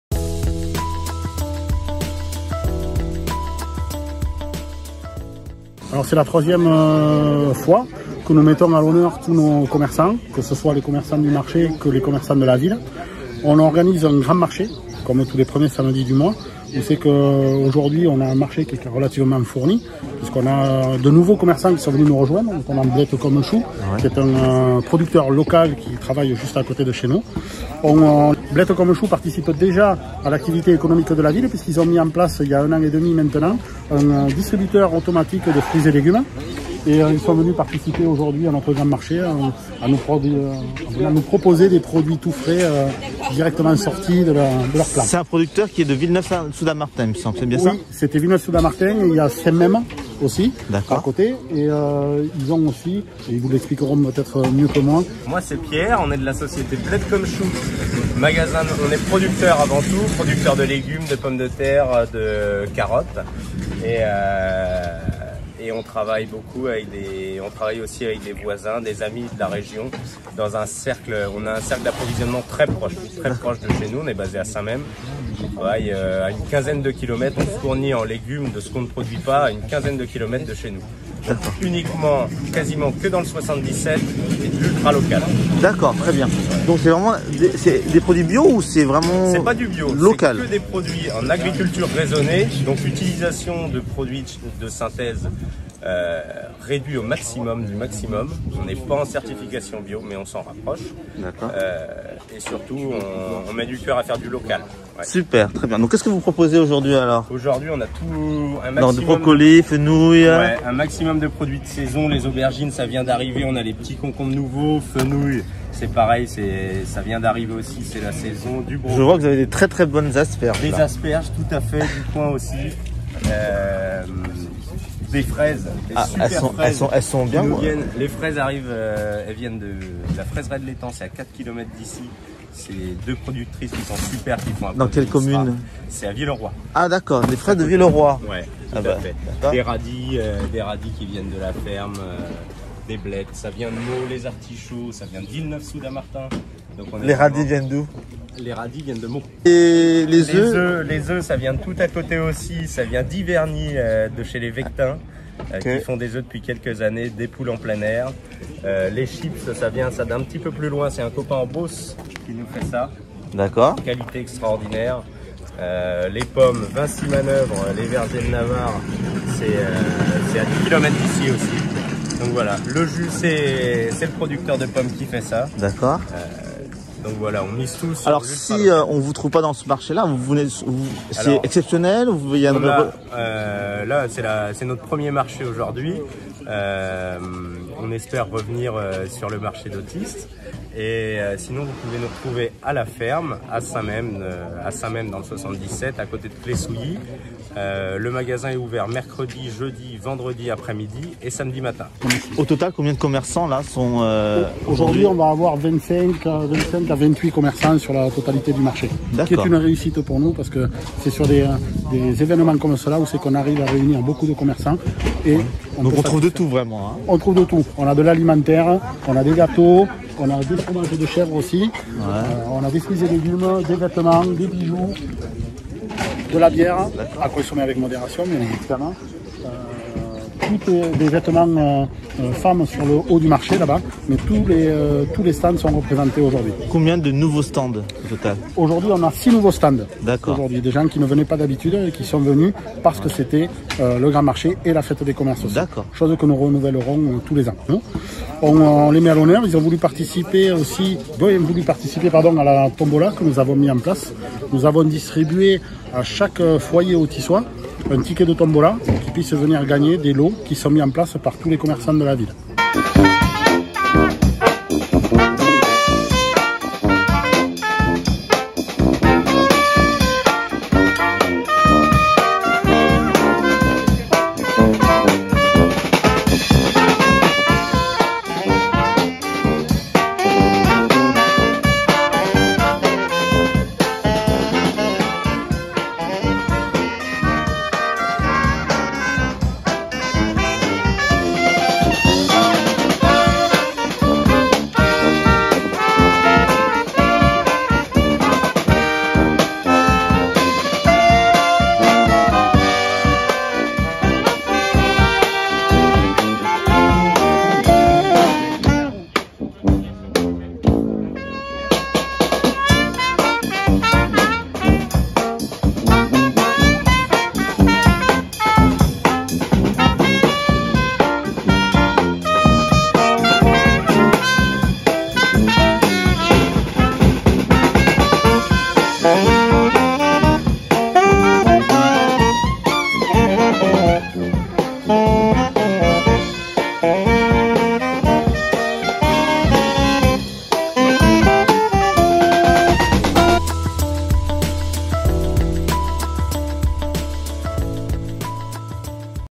Othis ► Blette comme Chou s’installe au marché avec des produits 100% locaux
Samedi 7 mai 2022 - Othis ► [Vidéo] Blette comme Chou s’installe au marché avec des produits 100% locaux
Blette comme Chou était l’un des six nouveaux commerçants du marché. Sébastien Dhom, adjoint au maire d’Othis chargé des activités économiques, déclare : «...